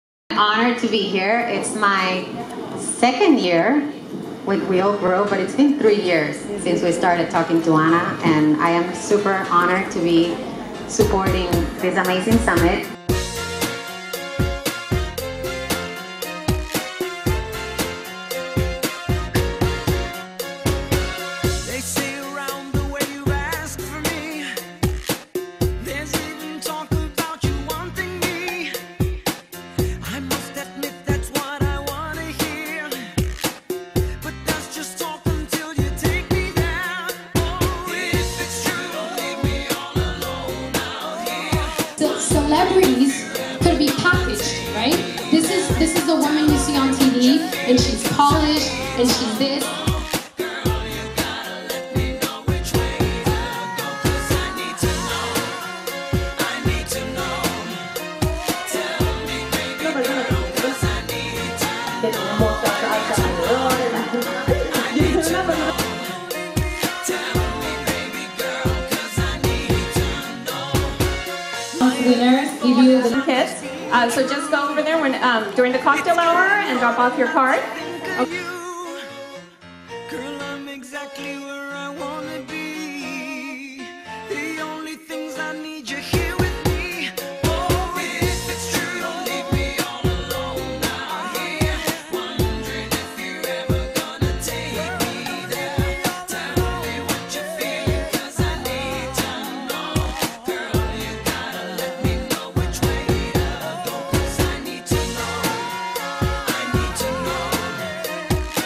We All Grow Summit Latina Network: featuring Alejandra Espinoza
Crafted Signature was able to film for Latin Celebrity, Alejandra Espinoza.

The WeAllGrow Summit in Miami featured Alejandra Espinoza on their panel among others discussing the power of marketing yourself in social media and blogs, and how to increase viewers and subscribers.